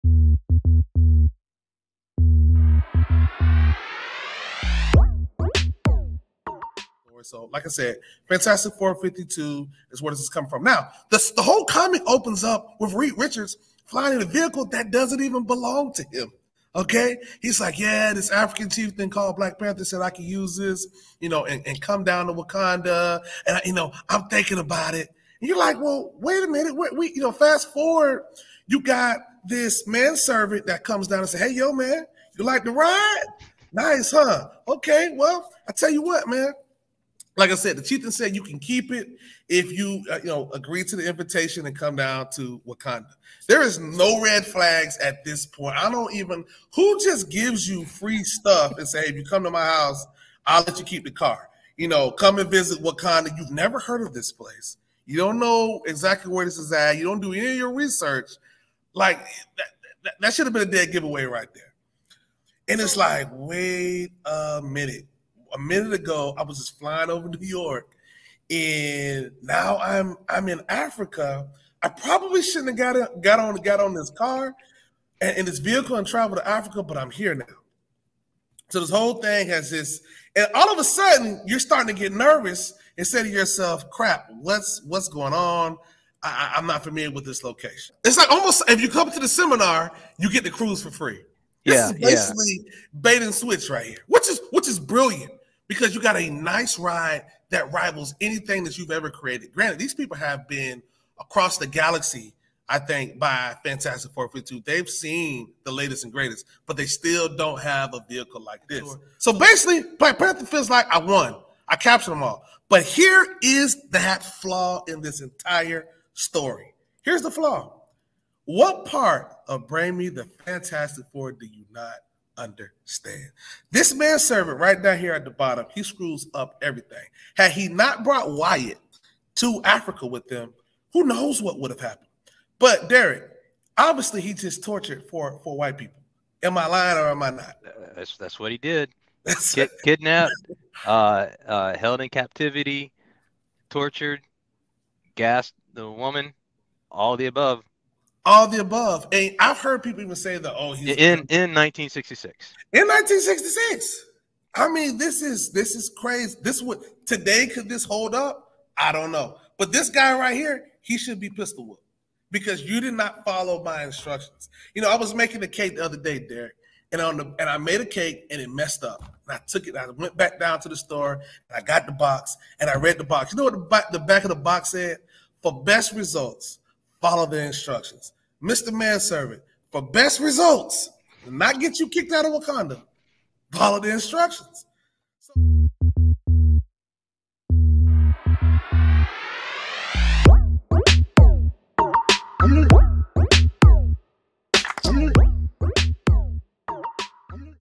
Quick Take: Fantastic Four 52 - Black Panther's Wild Intro into Comics
Most comic book fans know the first appearance of T'Challa was Fantastic Four #52. This video is a quick take from a previous video where Hershel gives a comedic review on just how crazy T'Challa's first appearance actually was.